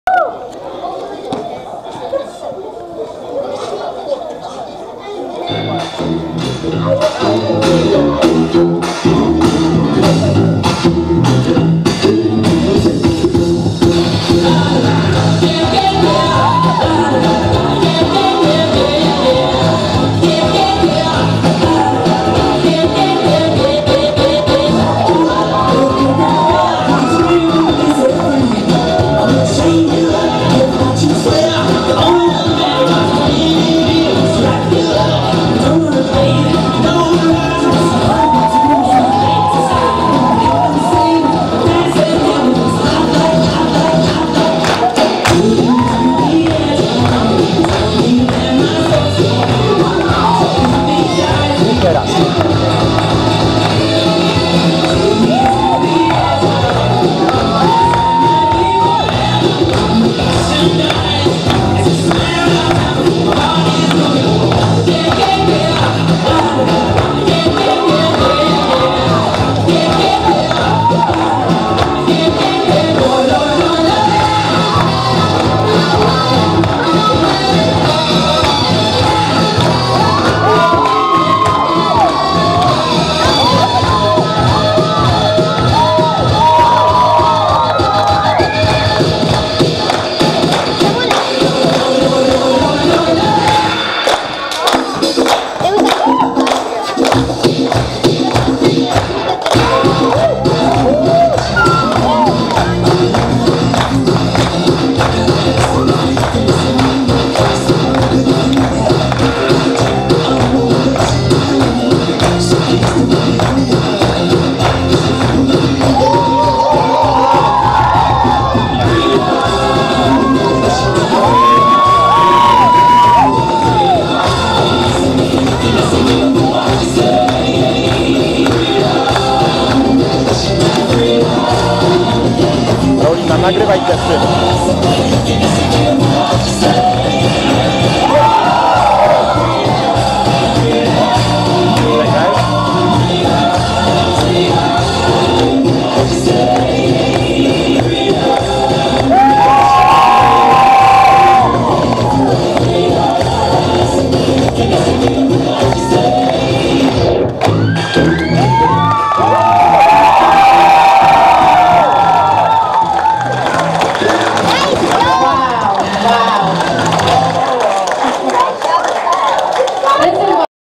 Viktor & Sasza Ania & Jacek USA Show FUN
Rock n Roll Acrobatic Boston Show